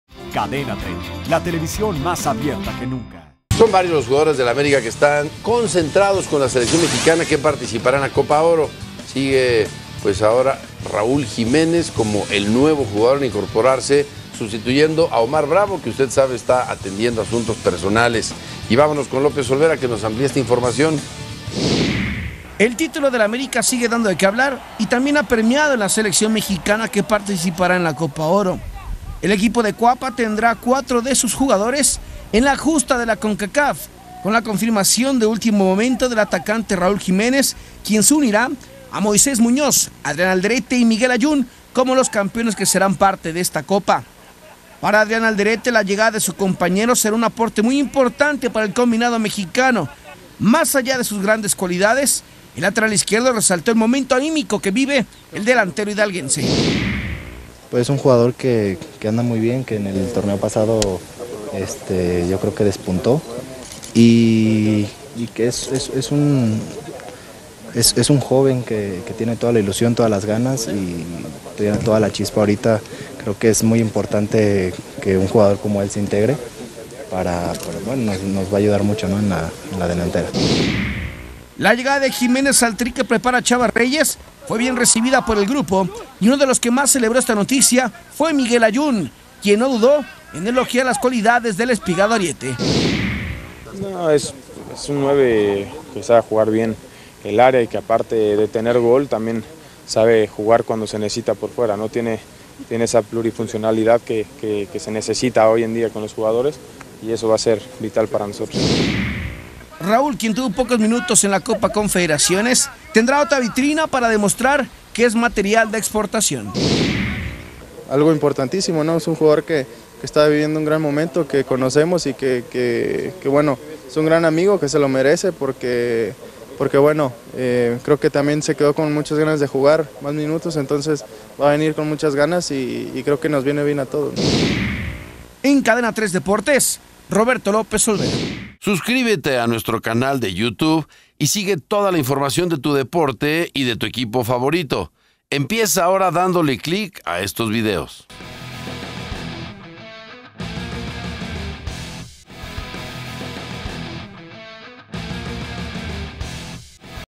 Raúl Jiménez se une al Tricolor Copa Oro; ya son 4 americanistas en el plantel de Chava Reyes
Raúl Jiménez se une al Tricolor Copa Oro; ya son 4 americanistas en el plantel de Chava Reyes 28/06/13

Las Águilas del América tendrán a cuatro de sus jugadores en la Copa Oro con la confirmación de último minuto del atacante Raúl Jiménez, quien se unirá a Moisés Muñoz, Adrián Aldrete y Miguel Layún como los campeones que serán parte de la justa de Concacaf.